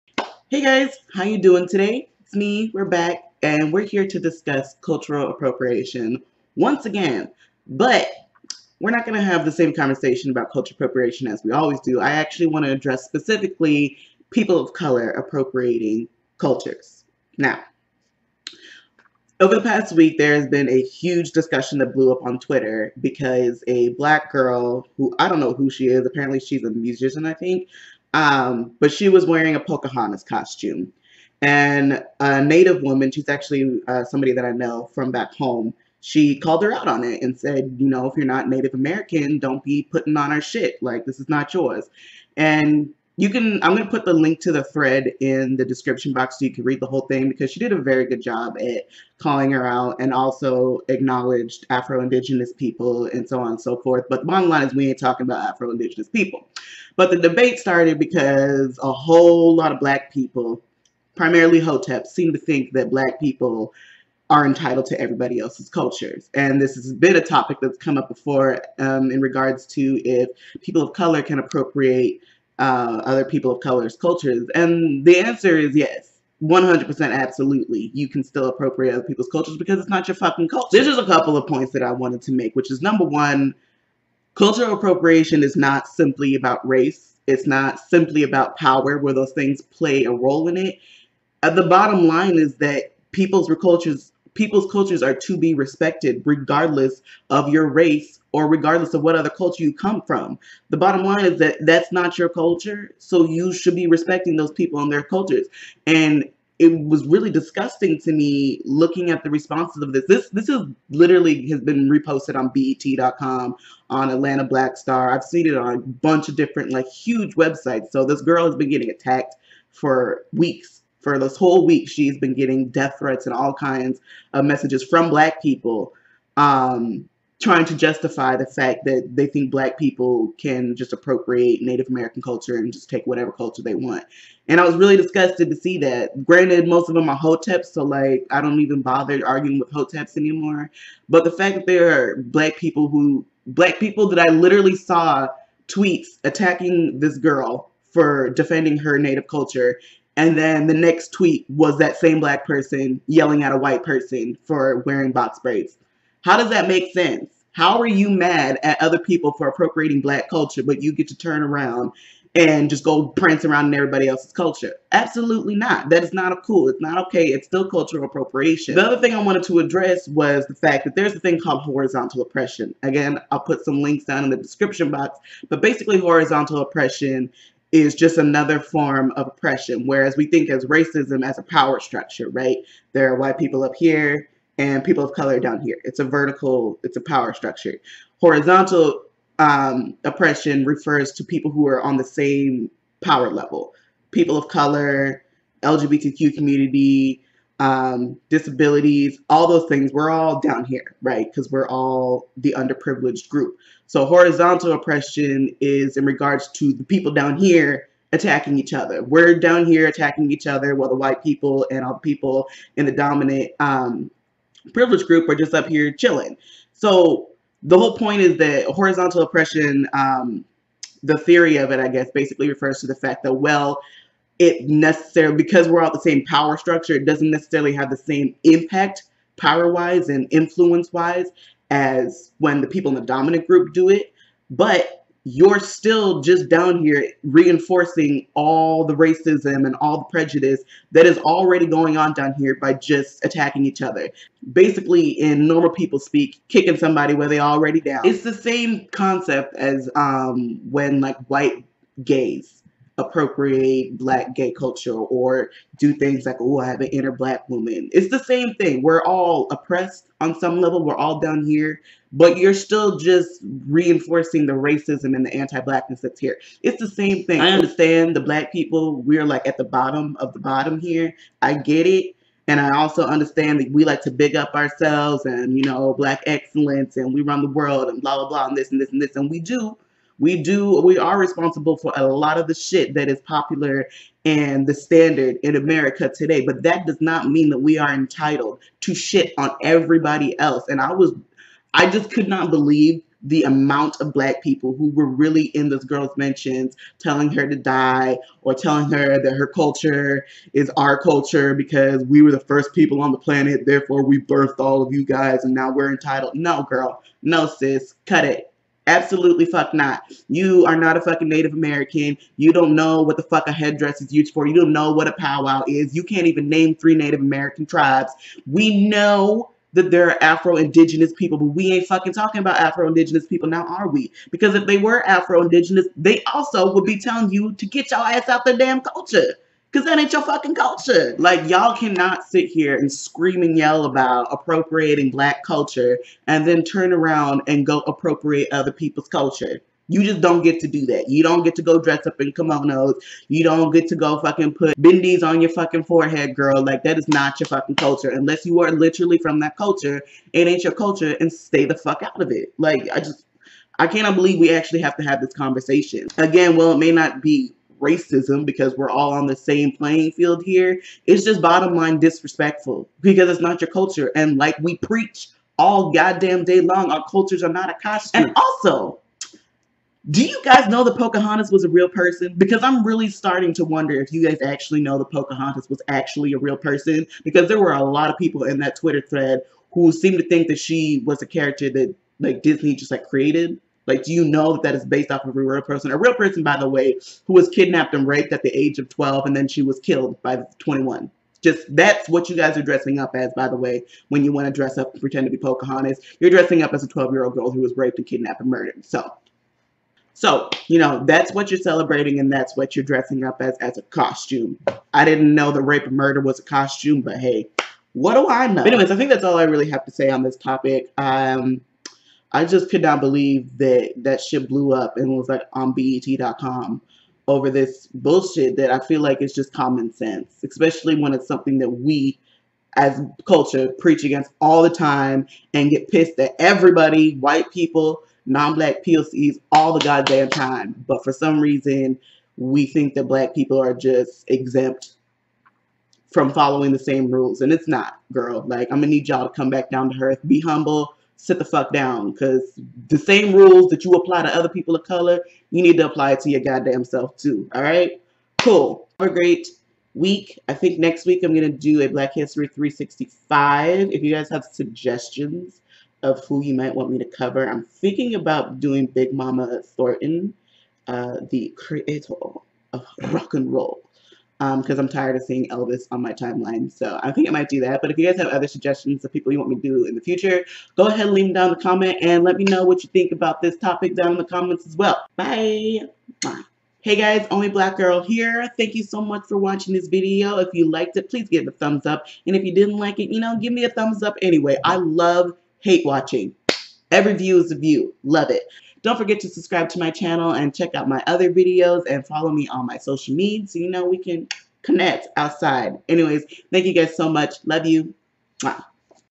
Can Black People Appropriate Other Cultures?
Debate on twitter sparked after a native woman called out a black woman wearing a Pocahontas costume, raising the question— black people can appropriate culture?

Also available on Stitcher, Google Play & iTunes
I'm Everywhere Like Wifi